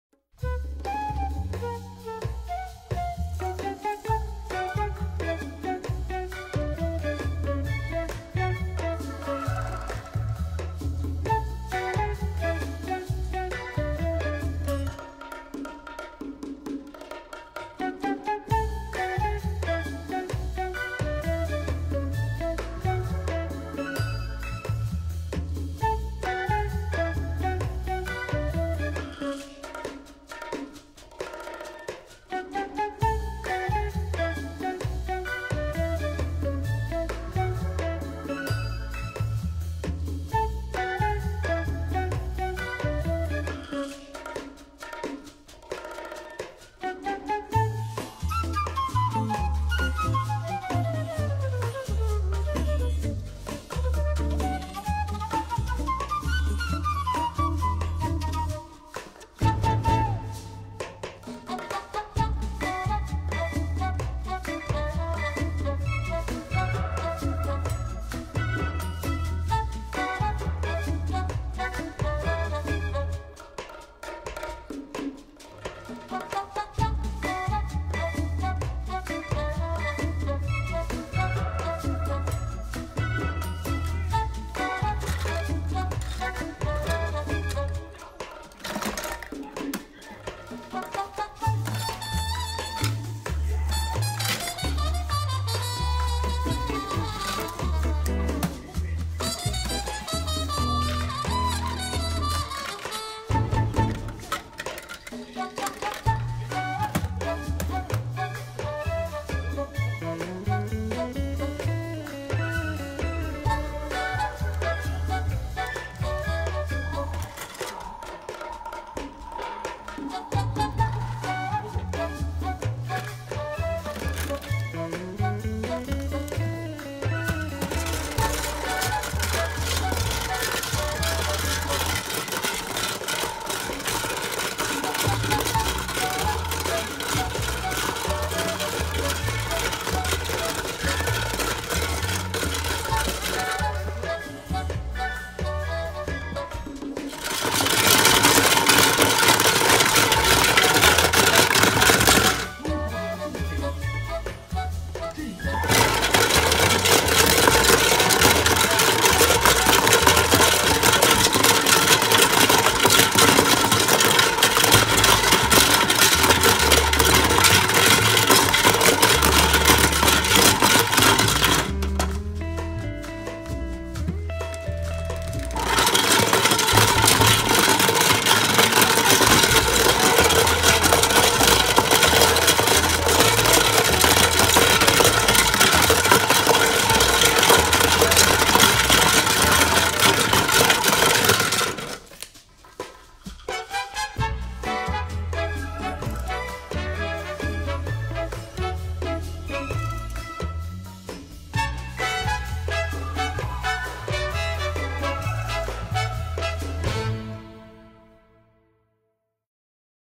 Washing Play-Doh Toys
Pre-washing of Play-Doh accessories in toy washing machine